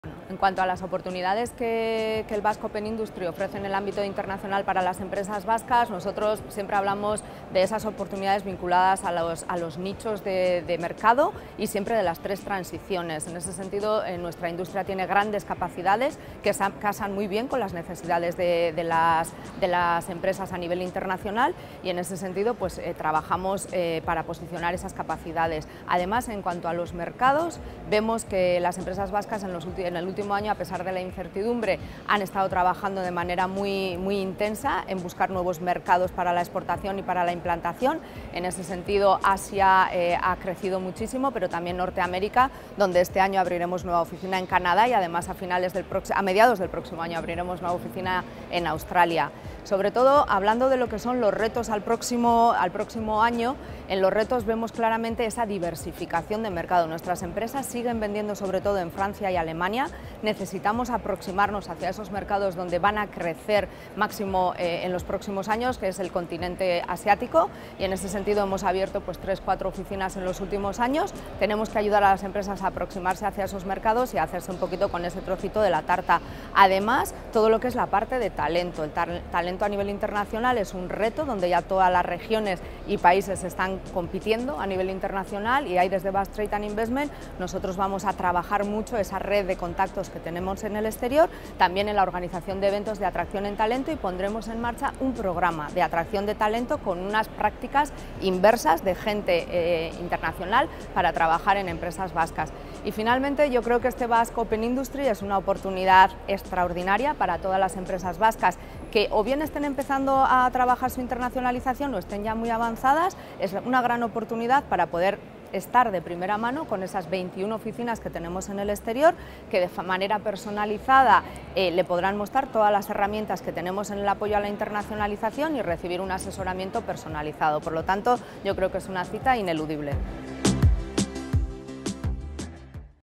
AINHOA ONDARZABAL: "Nuestra industria casa muy bien con las necesidades internacionales"
Durante la pasada edición del Basque Open Industry, Ainhoa Ondarzabal, Directora General de Basque Trade & Invetment, destaco que "nuestra industria tiene grandes capacidades, que casan muy bien con las necesidades de las empresas a nivel internacional". Así mismo, menciono que "las empresas vascas, a pesar de las incertidumbres, han estado trabajando de manera muy intensa en buscar nuevos mercados para la exportación y la implantación".

Las jornadas Basque Open Industry, celebradas el pasado 13 y 14 de noviembre, suponen un punto de encuentro para la Industria Inteligente vasca. La cita contó con más de 2200 inscritos y la participación de 158 empresas en un evento que contó con una exposición de 15.000 m2 y presencia de 24 países de todo el mundo. Así mismo, se trataron temas como la transición digital y medioambiental, los retos demográficos, la internacionalización de la industria vasca, la innovación y nuevas tecnologías de gran potencial como la Inteligencia Artificial.